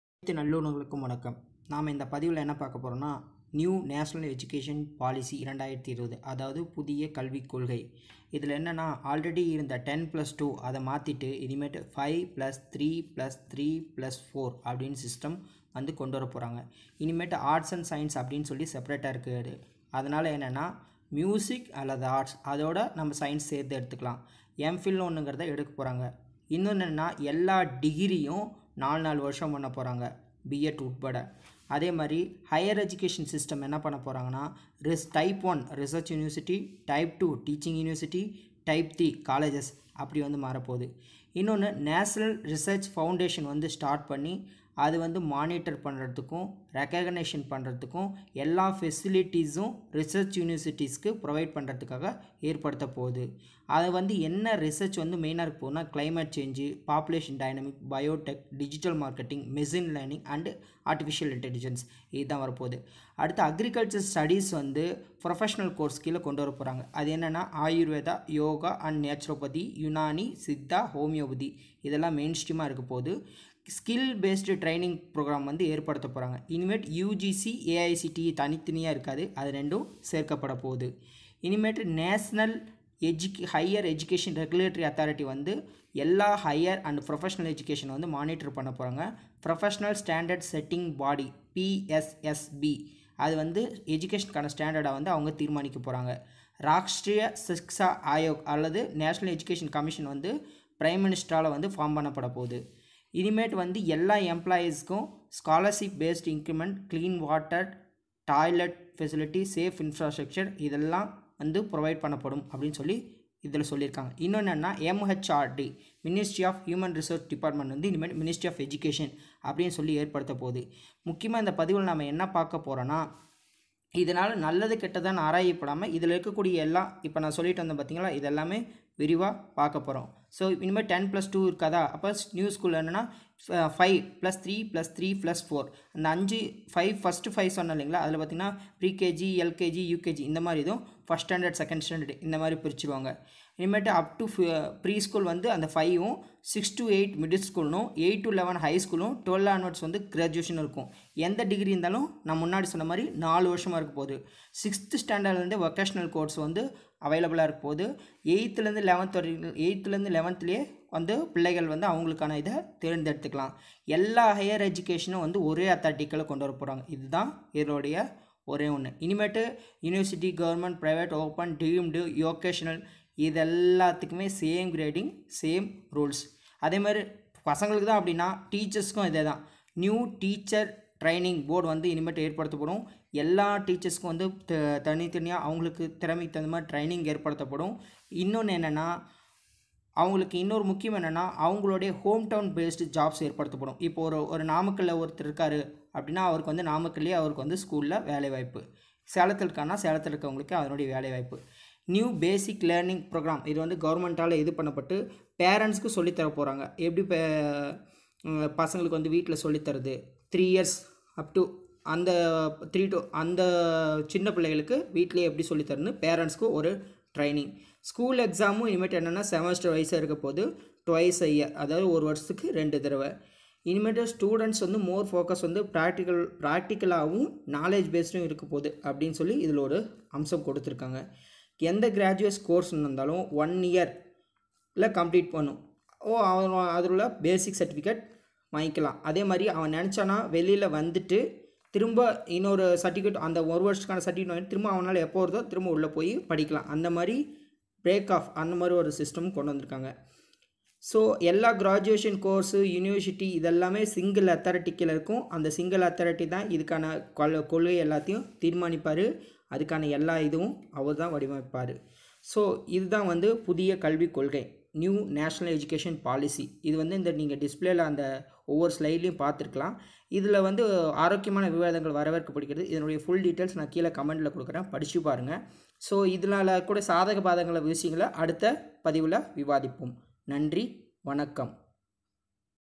NEP| NationalEducationPolicy2020 |புதியகல்விக்கொள்கை|SasidharanAnnamalai
1.New school structure will be 5+3+3+4

2.Upto 5 pre school, 6 to 8 Mid School, 8 to 11 High School , 12 onwards Graduation

3. Any Degree will be 4 years  No separation of Arts and Science: meaning you can elect both Music/arts along with science.

4. National Research Foundation to be Started helping in recognition, financing, mentoring and providing facilities to all reserach an teaching universities.

5. 6th std onwards vocational courses available,from 8th to 11 students can choose subjects

6. All graduation course will have major and minor
Example - science student can have Physics as Major and Music as minor also. Any combination he can choose

7. All higher education will be governed by only one authority.
Higher Education System: Institutional Restructuring to be done in 3 levels.

Type1 -Research Universities
Type 2- Teaching Universities
Type 3- Colleges

8. UGC AICTE will be merged.
9.All University government, private, Open, Deemed, Vocational etc will have same grading and other rules.
10.New Teacher Training board will be setup for all kinds of teachers in country, no state can change

11.First time ever, Transgenders to be given admission in all schools.

Focus on urban poor families, tribal education to be emphasized.

12. Same level of Accreditation to any collage , based on its rating collage will get autonomous rights and funds.
13. New Basic learning program will be created by government for parents to teach children upto 3 years in home and for pre school 3 to 6
14. Multiple entry and exit from any course
15. Credit system for graduation for each year student will get some credits which he can utilize if he takes break in course and come back again to complete course
16. All schools exams will be semester wise twise a year
17. The syllabus will be reduced to core knowledge of any subject only
18. More focus on student practical and application knowledge
19. For any graduation course if student complete only one year he will get a basic certificate, if he complete two years then he will get Diploma certificate and if he complete full course then he will get degree certificate. So no year of any student will ve vested if he break the course in between.
20.All the graduation course feed of all Universities will be govern by single authority with capping on each course.

Education in mother tongue to be given recognition and prominence.